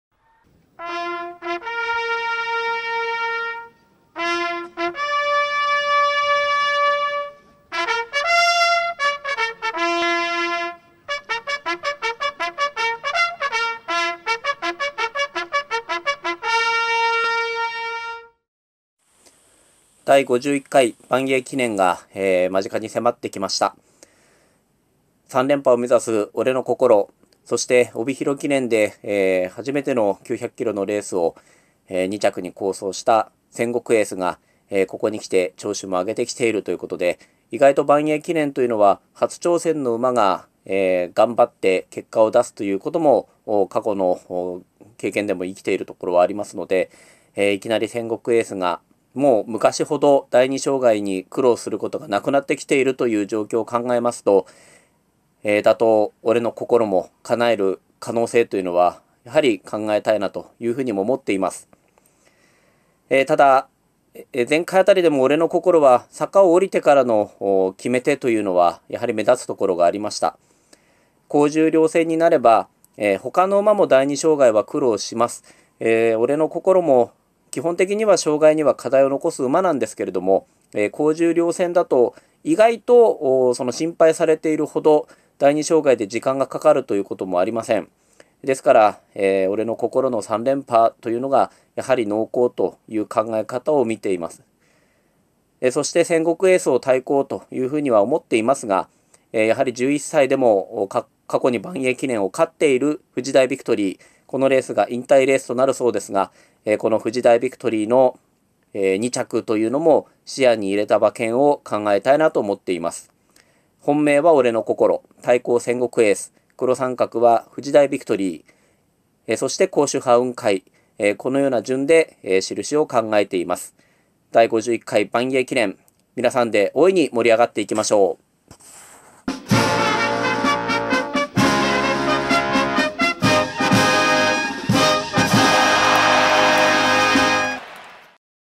カウントダウンセルフィー古谷剛彦さん Vol.6
ばんえい記念2019カウントダウン予想の第6弾は、楽天競馬スペシャルアドバイザーの古谷剛彦さん。
ばんえい記念は意外と初挑戦組が好走するパターンがあるのです。
帯広記念で初の900kgも2着好走し障害も苦にしなくなった初挑戦のあの馬が、三連覇をめざす王者の牙城を崩せるか。
ここが引退レースとなる古豪も侮れません。

【出演・制作協力】
　古谷　剛彦

【制作】
　ＯＰｒｏｊｅｃｔ株式会社

【制作協力】
　ばんえい十勝

【制作著作】
　楽天競馬